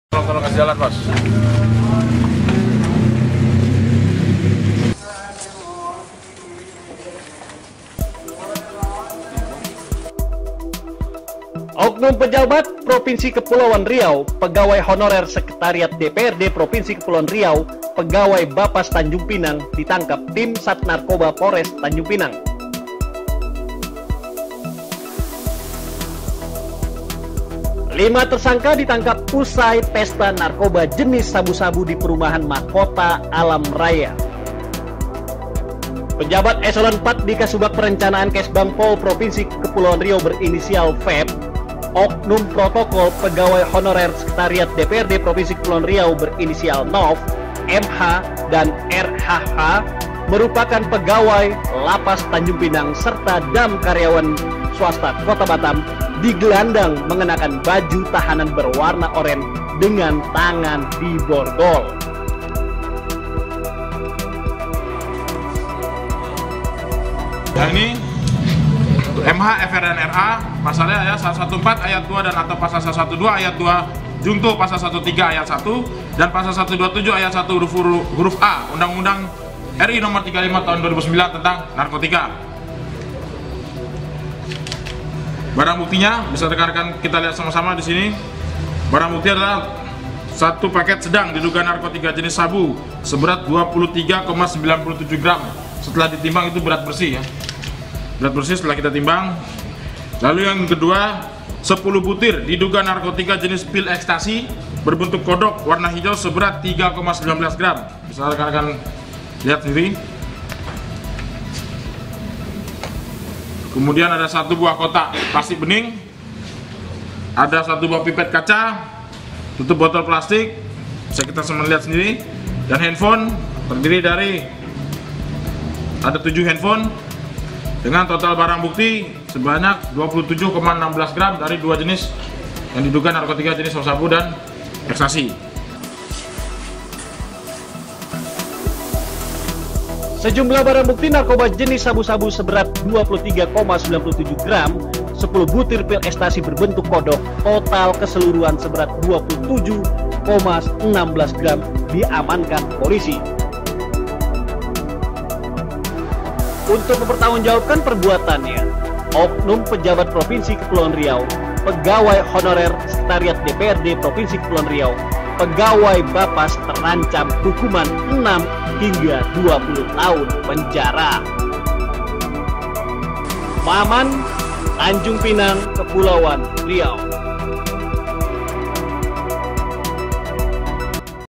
3 PEJABAT DAN OKNUM PROTOKOL SEKWAN DPRD KEPRI PESTA SABU DITANGKAP
TANJUNGPINANG, GURINDAM. TV -- OKNUM PEJABAT PEMPROV KEPRI, PEGAWAI HONORER SEKWAN DPRD  KEPRI, PEGAWAI BAPAS TANJUNGPINANG DI TANGKAP TIM SATNARKOBA POLRES TANJUNGPINANG.

LIMA TERSANGKA DITANGKAP USAI PESTA NARKOBA JENIS SABU-SABU DI PERUMAHAN MAHKOTA ALAM RAYA BLOK GLADIOL 3 NO 18A, JALAN HANG LEKIR TANJUNGPINANG KEPULAUAN RIAU.